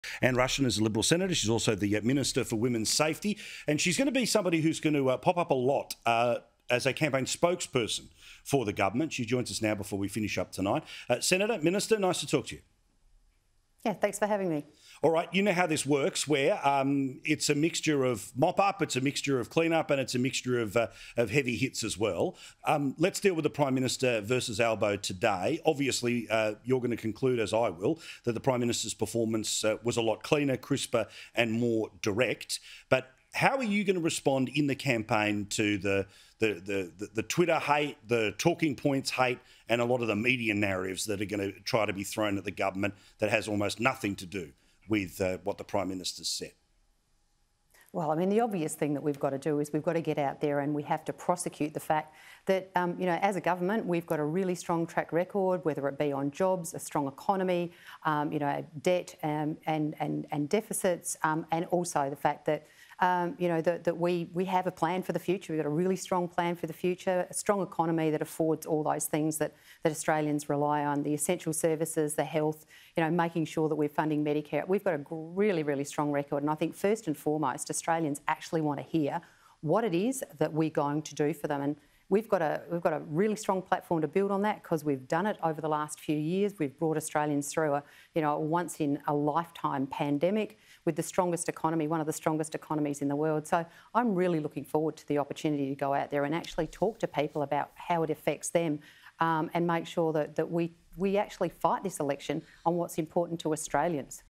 'Obvious thing' the Liberal party needs to do is 'prosecute' government's strong track record
Liberal Senator Anne Ruston says the obvious thing to do is for the party to go out and prosecute the fact the government has a “really strong track record”.

“Whether it be on jobs or a strong economy, you know, debt and deficits and also the fact that we have a plan for the future, we got a really strong plan for the future,” Ms Ruston told Sky News host Paul Murray.

“We’ve got a really, really strong record, and I think first and foremost, Australians actually want to hear what it is that we’re going to do for them, and we’ve got a really strong platform to build on that.

“I’m really looking forward to the opportunity to go out there and actually talk to people about how it affects them and make sure that we actually fight this election on what’s important to Australians.”